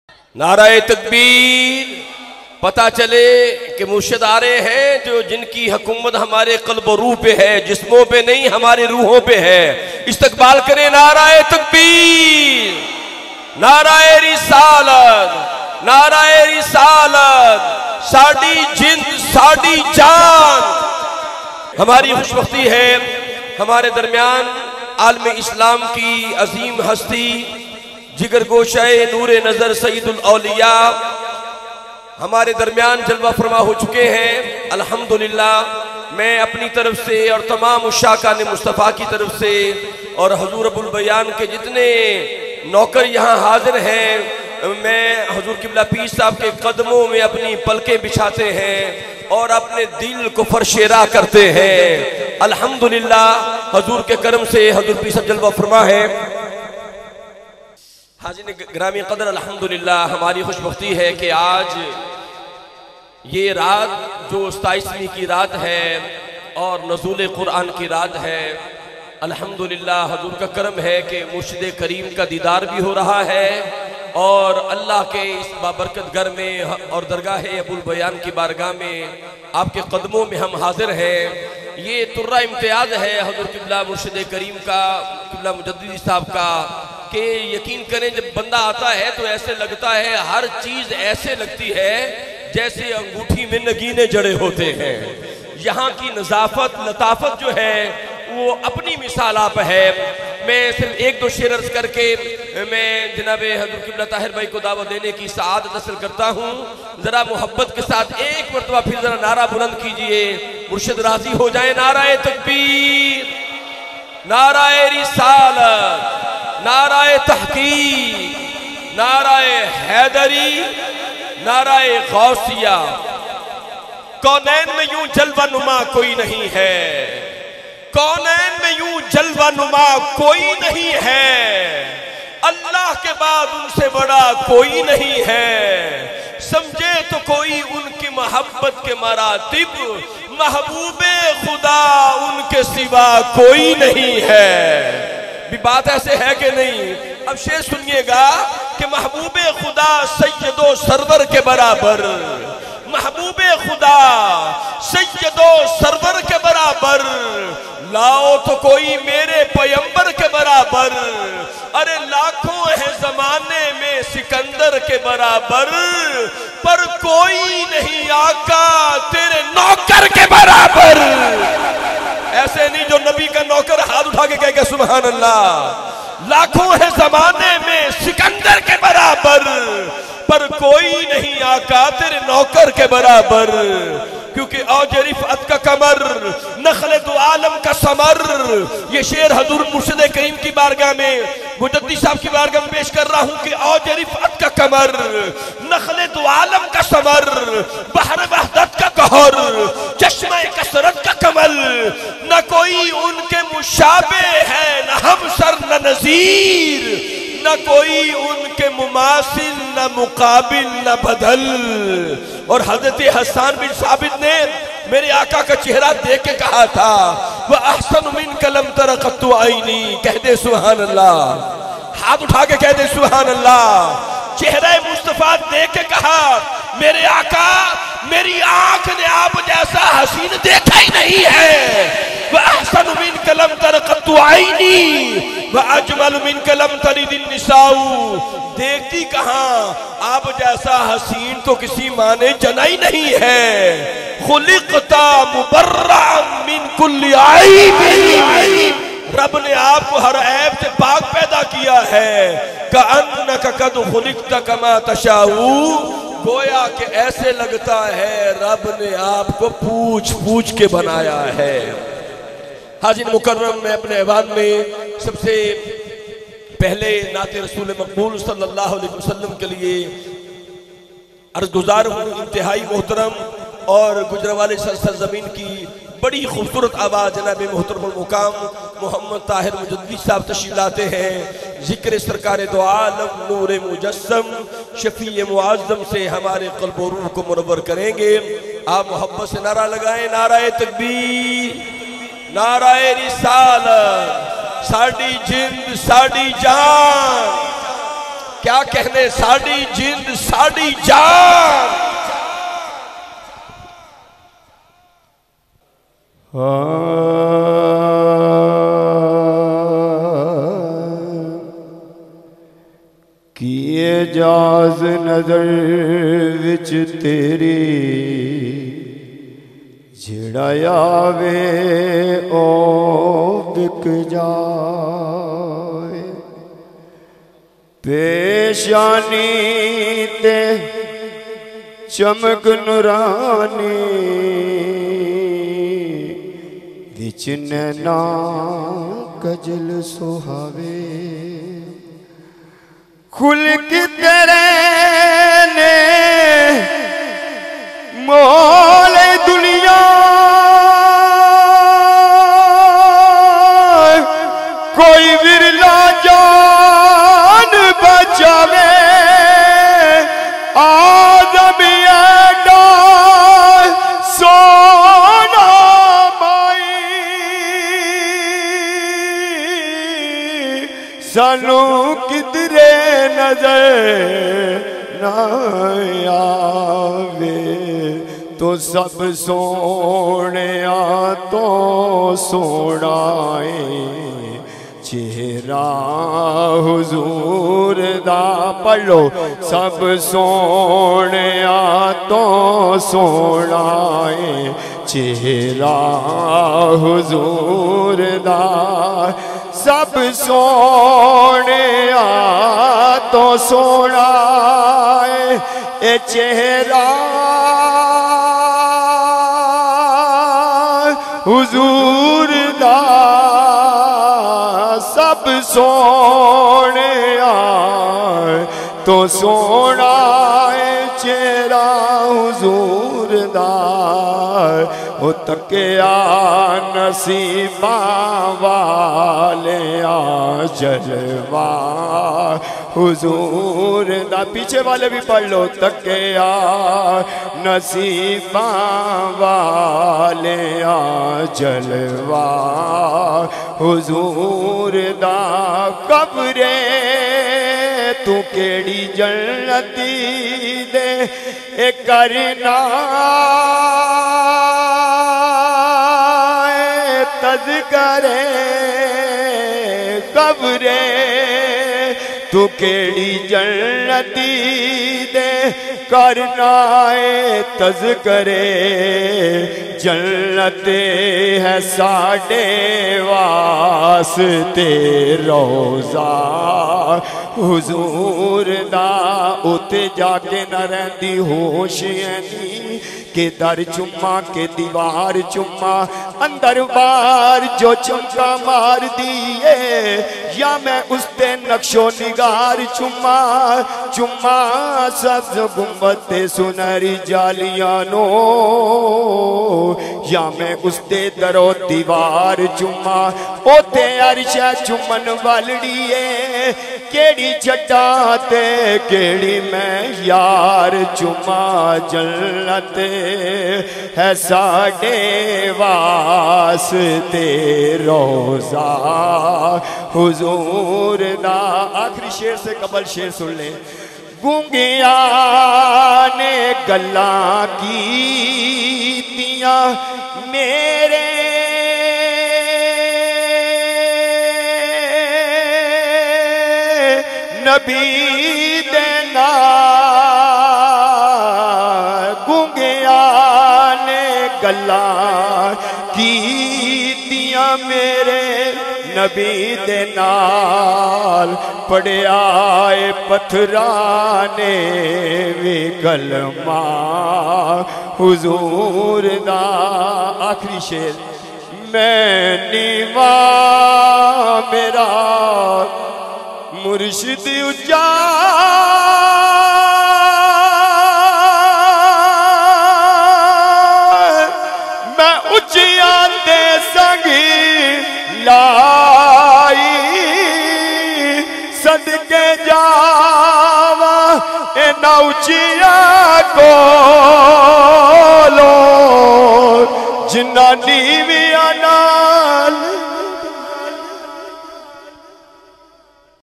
Tahir Mehmood Mujaddadi , at Jashn e Nazool e Quran 2019
محفلِ جشنِ نزولِ قرآن برموقع 27 رمضان المبارک ۔
 زیرِسرپرستی : عالمی مبلغِ اسلام جانشین ابوالبیان صاحبزادہ پیر محمد رفیق احمد مجددی مدظلہ ۔
بتاریخ : 1 جون بروزہفتہ 2019
بمقام : درگاہ حضرت ابوالبیانؒ 121 - بی ماڈل ٹاؤن گوجرانوالہ ۔
زیرِ اہتمام : عالمی ادارہ تنظیم الاسلام ۔